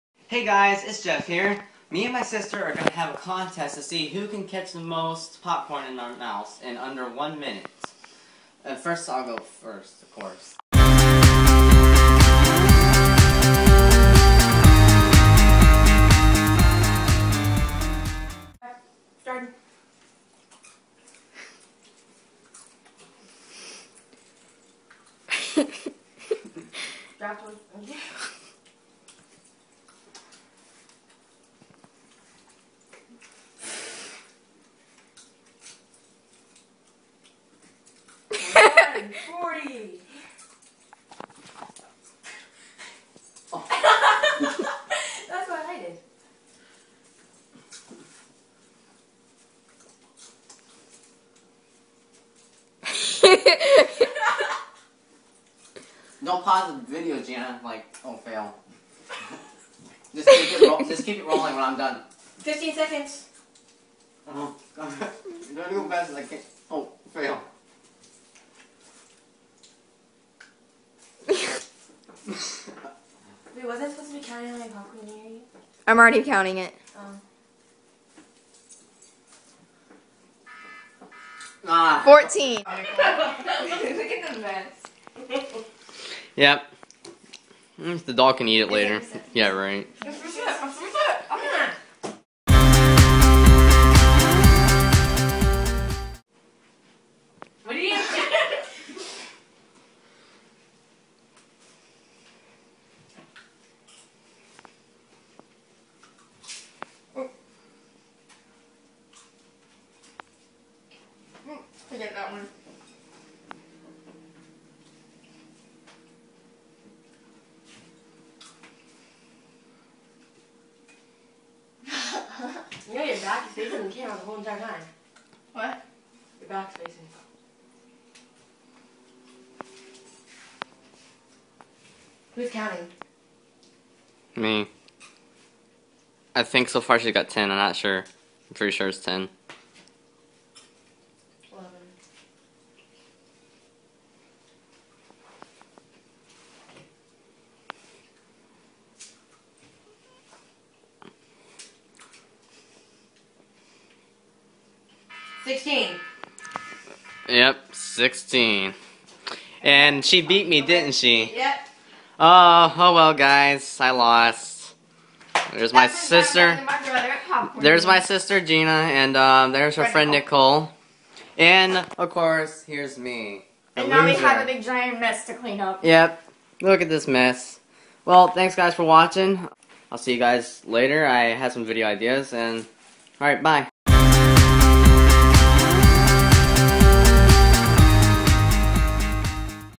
Popcorn catching contest?
See who can catch the most popcorn under one minute.